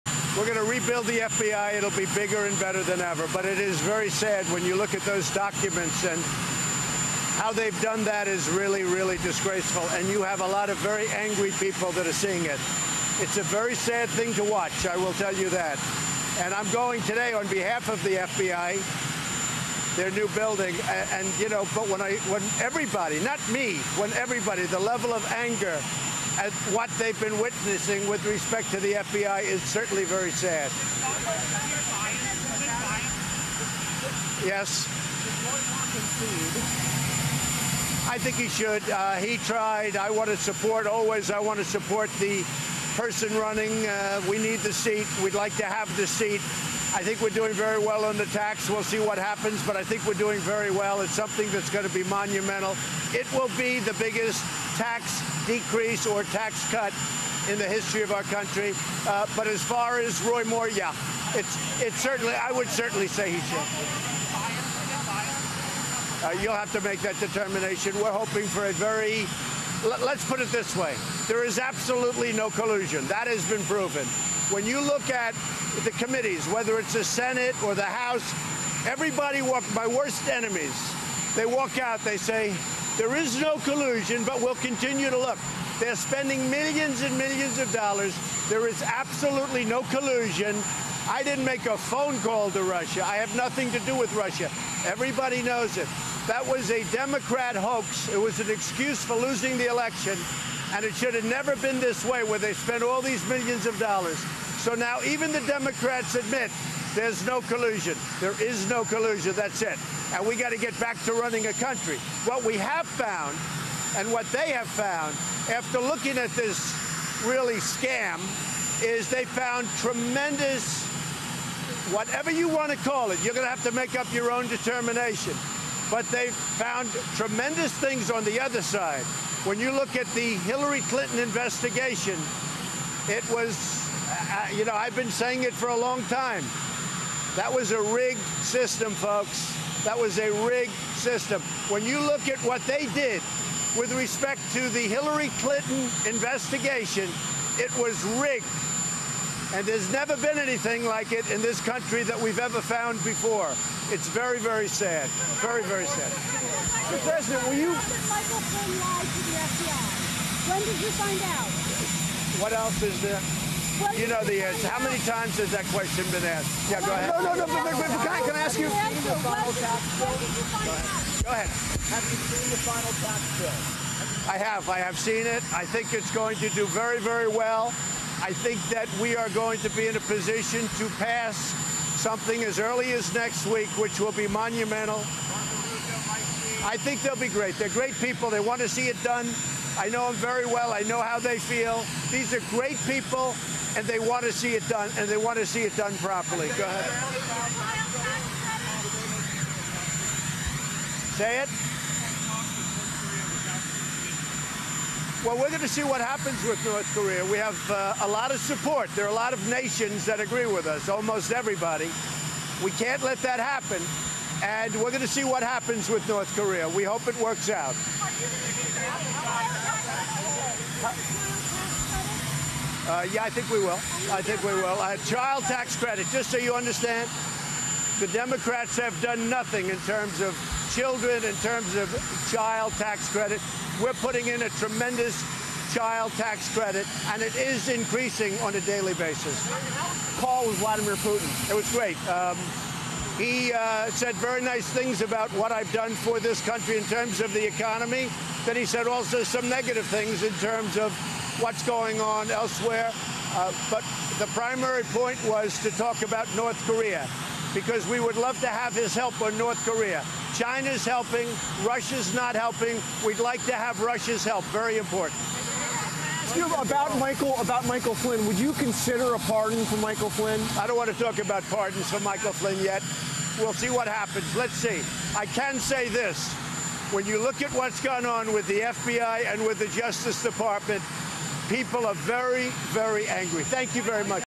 Trump: Moore Should Concede Election; 'We'll See' on Michael Flynn Pardon
Trump speaks outside White House on Roy Moore, Russia, Michael Flynn.